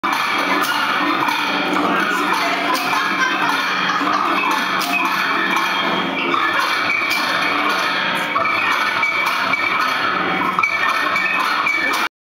Cranks Host Guest Conductors
Garden of Memory at Chapel of the Chimes in Oakland, summer solstice 2010. We asked guest conductors to direct us.